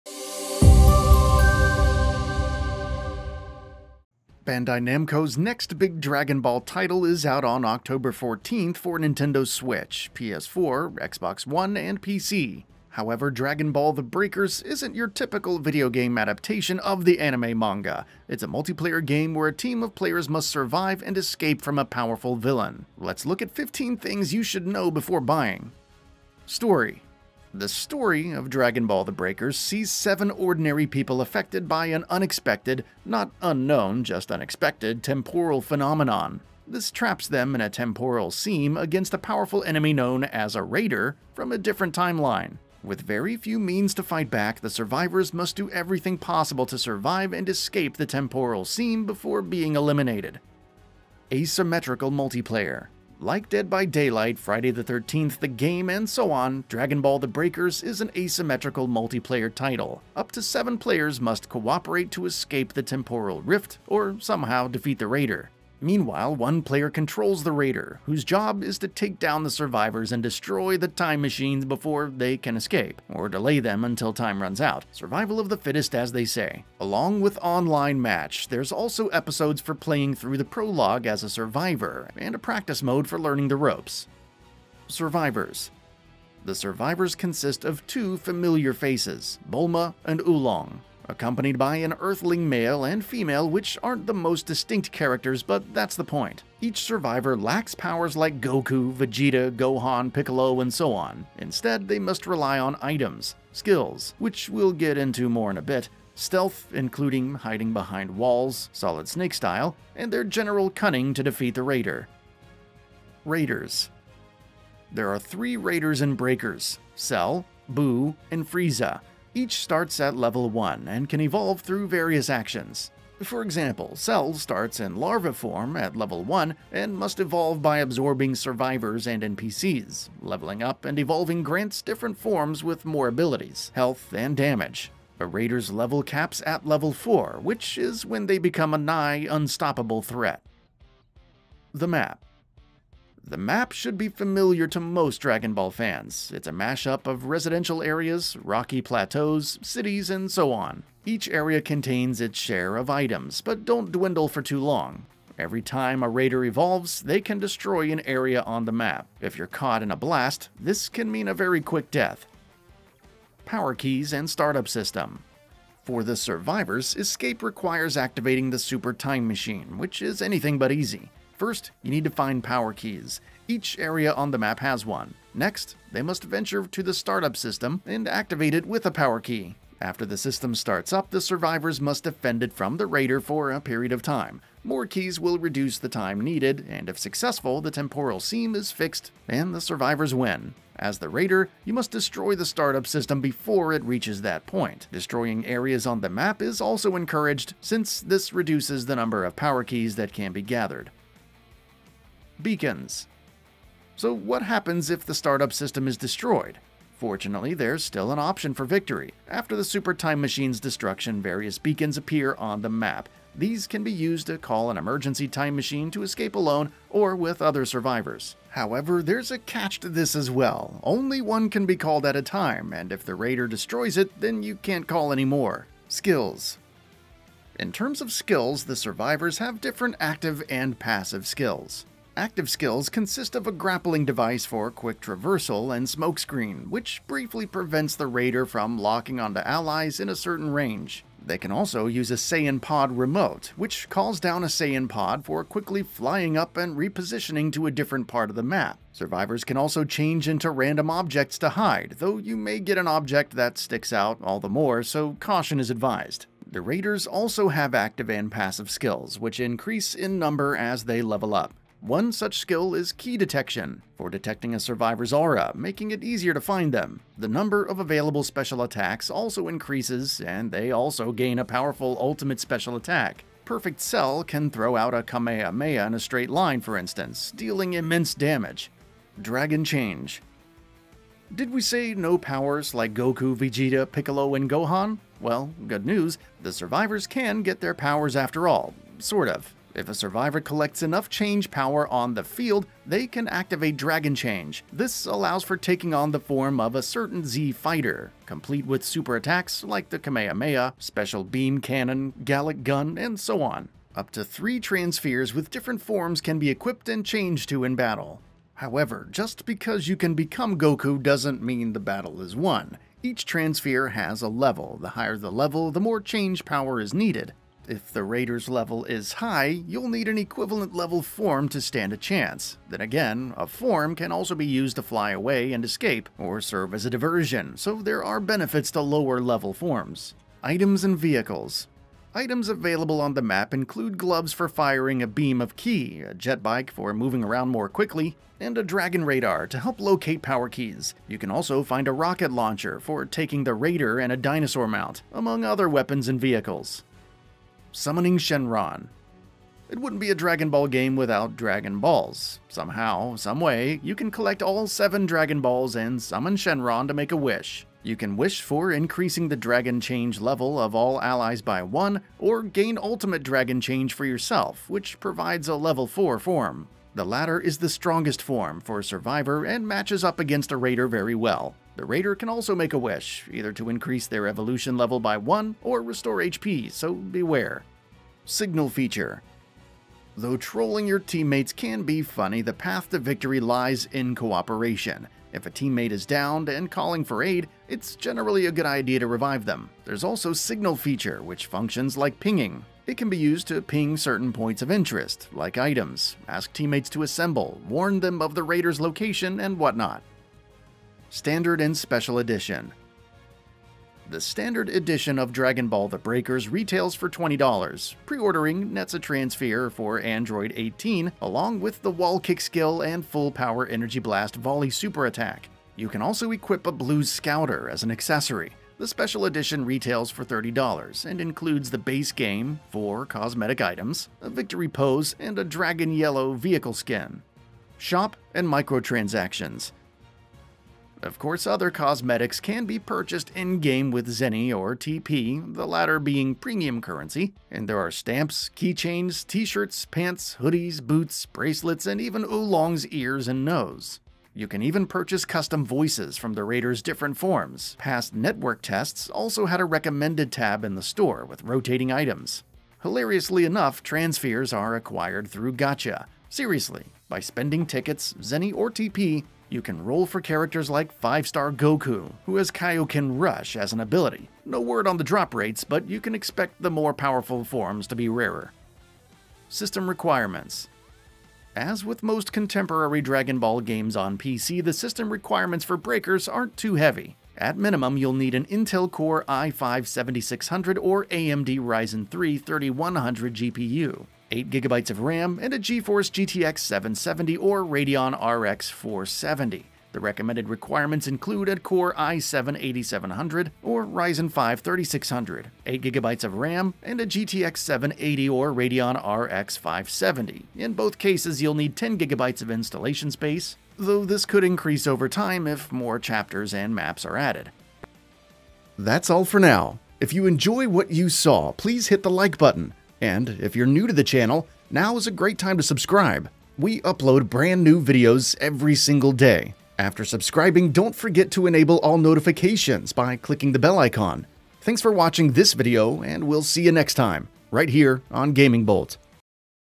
Dragon Ball The Breakers - 15 Things You Need To Know Before You Buy
Bandai Namco's next big Dragon Ball title is out on October 14th for Nintendo Switch, PS4, Xbox One, and PC. However, Dragon Ball: The Breakers isn't your typical video game adaptation of the anime/manga.

It's a multiplayer game where a team of players must survive and escape from a powerful villain. Let's take a look at 15 things you should know before buying.